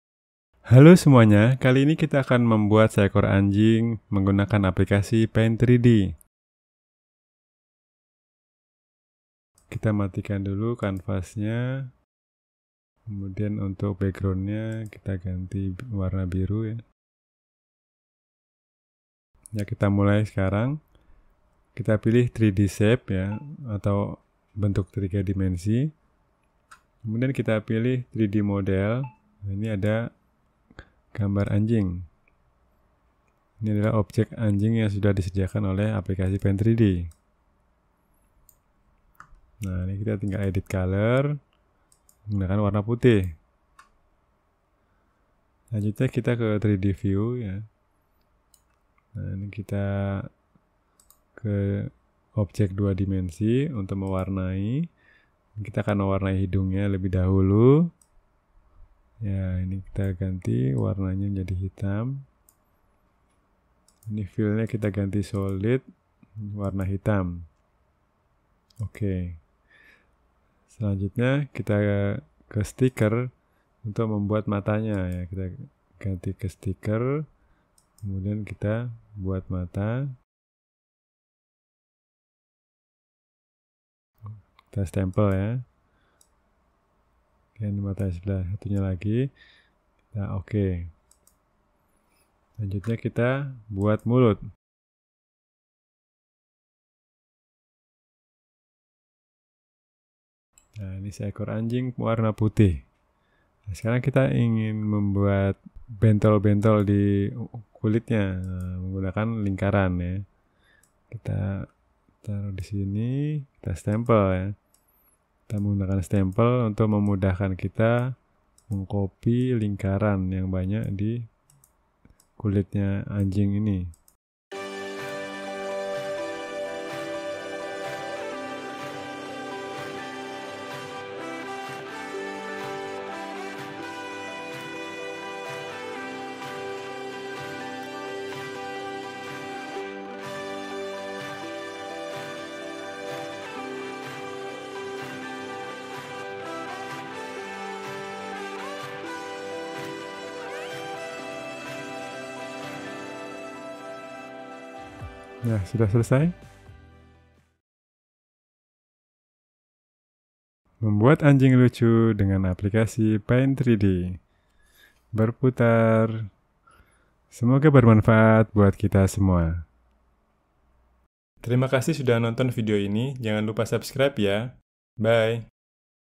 Menggambar Anjing Lucu dengan Paint 3D
Tutorial cara menggambar anjing lucu dengan paint 3d. Video ini menjelaskan cara membuat gambar anjing lucu dengan aplikasi Microsoft Paint 3D. Menggambar anjing dengan paint 3d sangat mudah.